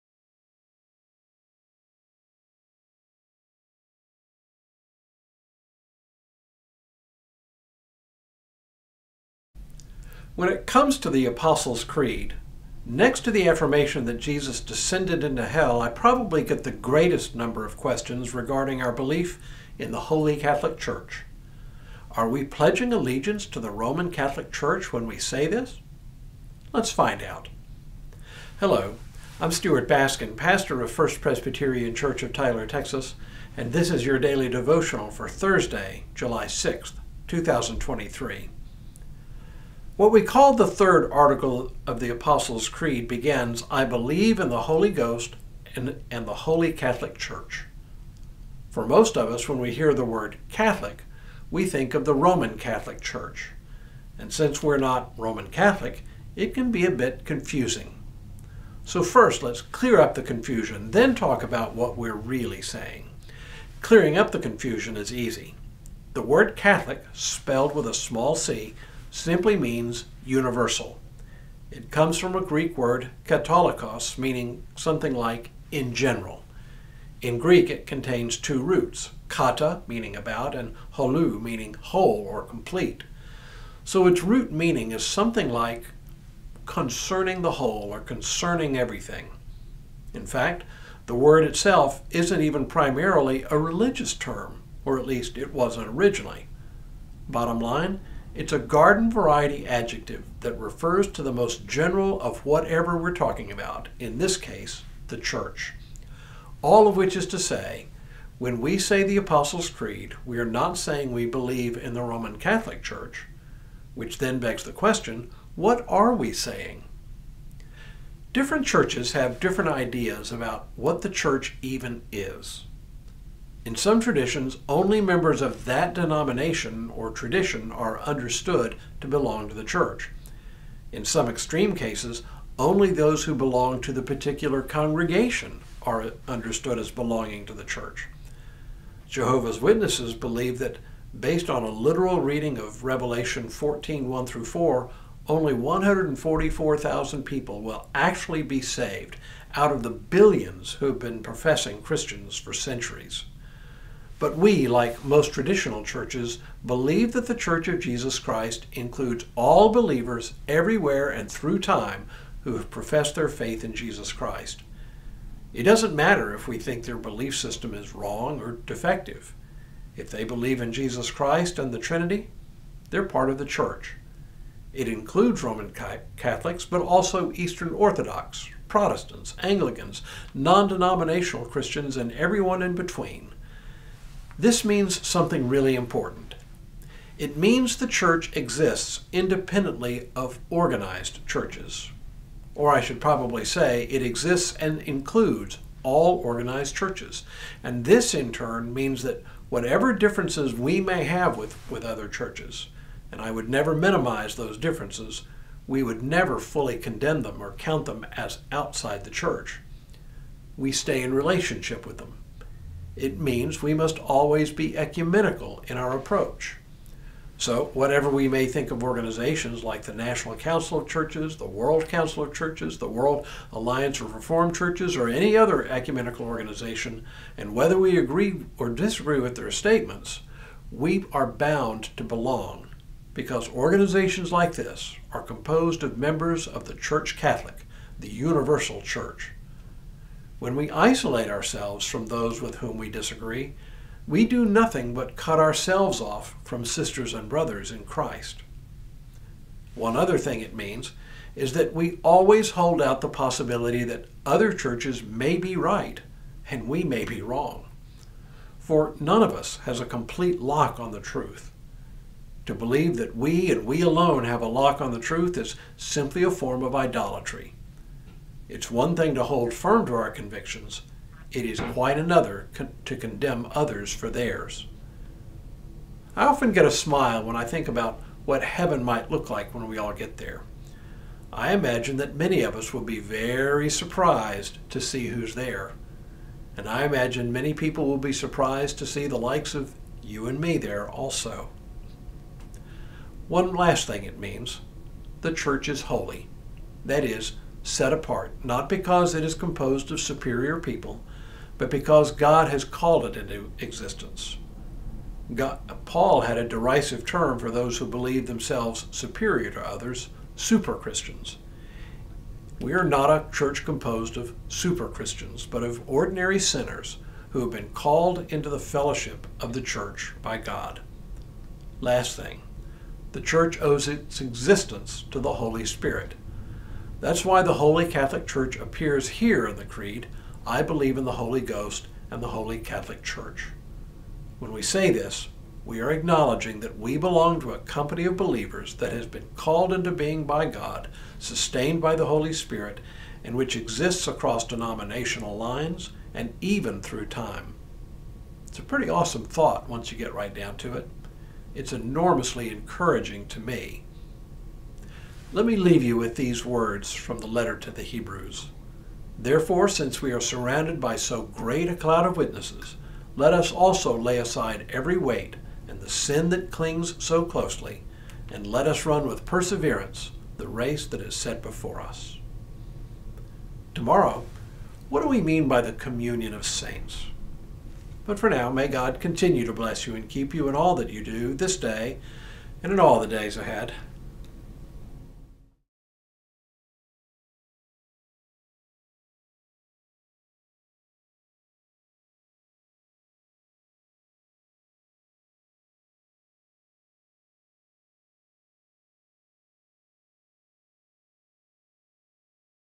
The Holy Catholic Church | FPC Tyler | Daily Devotional | July 6, 2023
Catholic? Or catholic? Join us for this daily devotional by Dr. Stuart Baskin, Pastor of First Presbyterian Church of Tyler, Texas, for Thursday, July 6, 2023. The text is Hebrew 12:1.